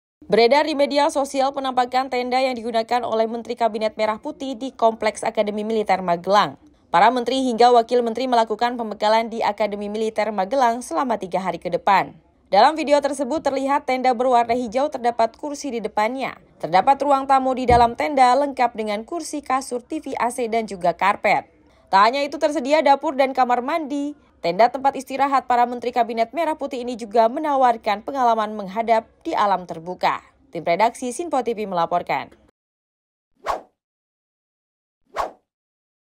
Tenda Yang Digunakan Para Menteri Kabinet Merah Putih
Para Menteri hingga Wakil Menteri melakukan pembekalan di Akmil Magelang selama 3 hari.